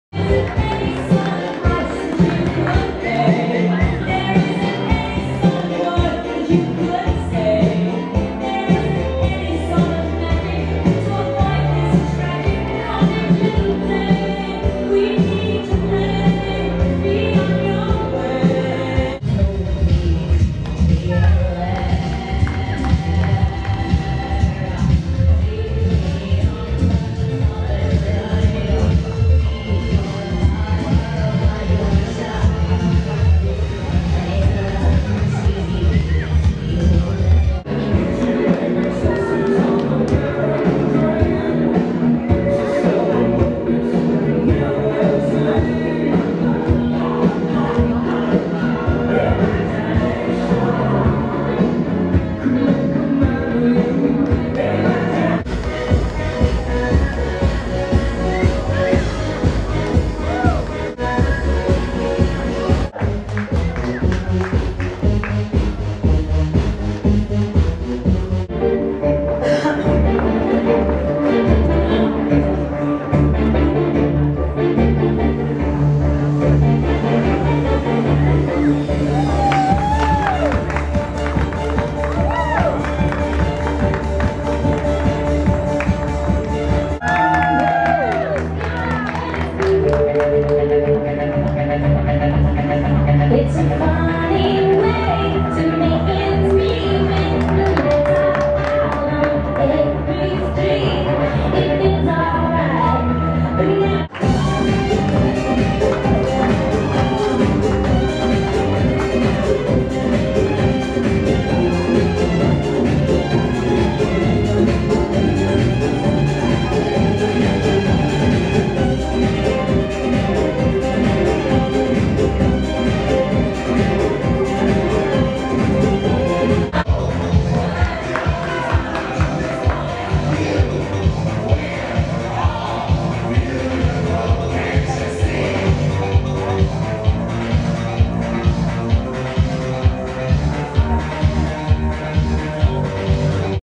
Celebrity Fashion Rocks
Eva modelling for Cloud 9 Children's Charity Teckels Animals Sanctuaries and K9-Angels (who rescue and find homes for stray and abandoned dogs) at the recent Celebrity Fashion Rocks event.
Clothes provided by SheuniqueVintage, Jungle Boutique, Bridal Studio, Cirencester Design Students, Lola, Fishhead Hats, HMH Couture, The House of Hannigan and Ali Faye.
The event was organised by Cutting Shed Russell Nurding and Willie Carson.